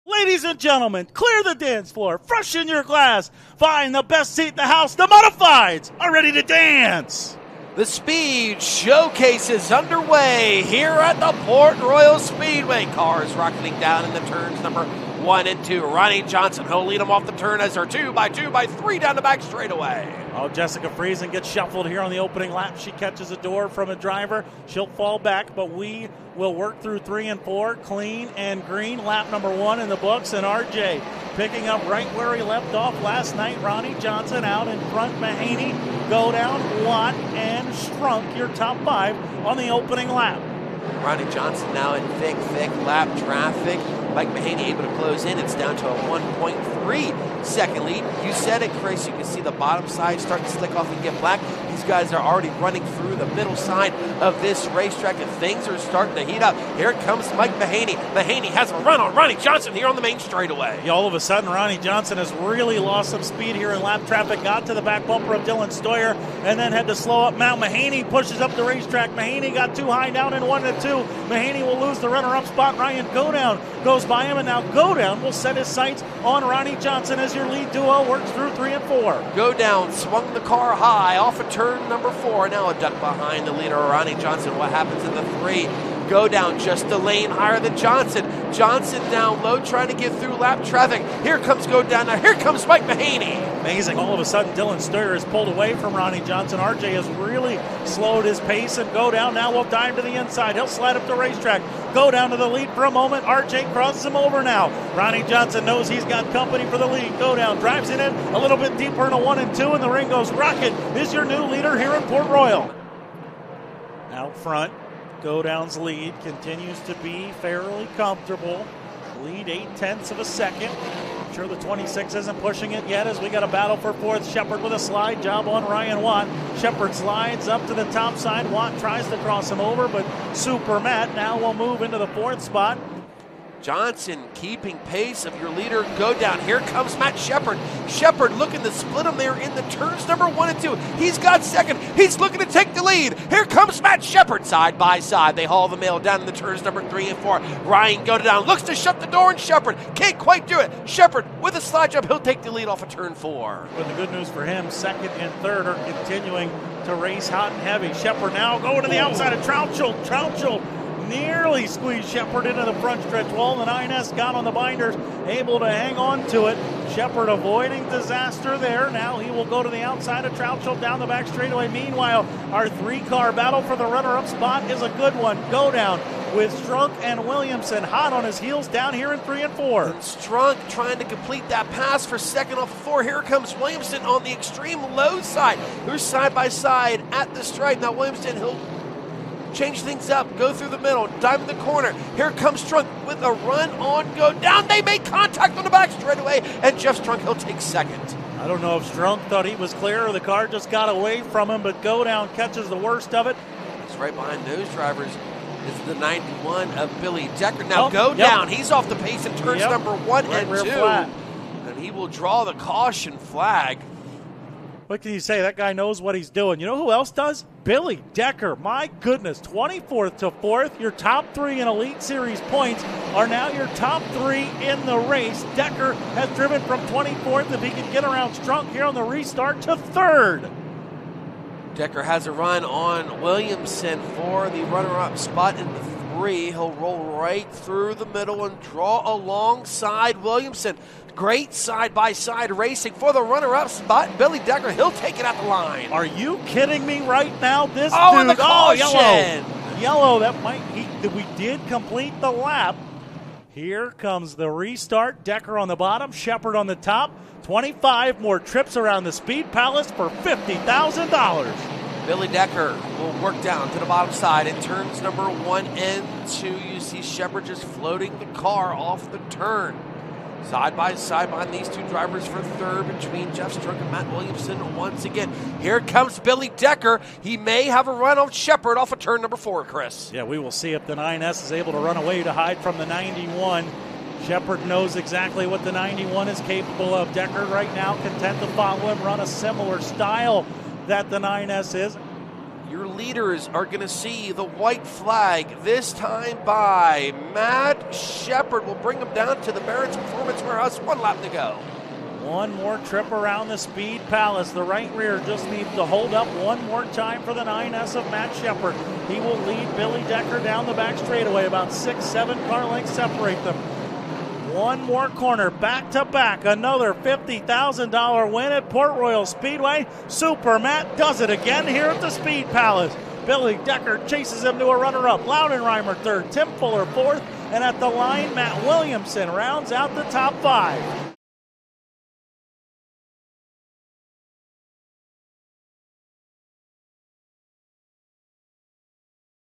$50,000-To-Win Big Blocks | 2024 STSS Speed Showcase at Port Royal Speedway
The Big Block Modifieds battle for $50,000 in the Short Track Super Series 'Speed Showcase' on Saturday, March 16, 2024.

Ryan Watt, Ronnie Johnson, Ryan Godown, Mike Mahaney and Mat Williamson captured Friday's qualifiers and will contend against defending Speed Showcase winner Matt Sheppard in the 75-lap feature.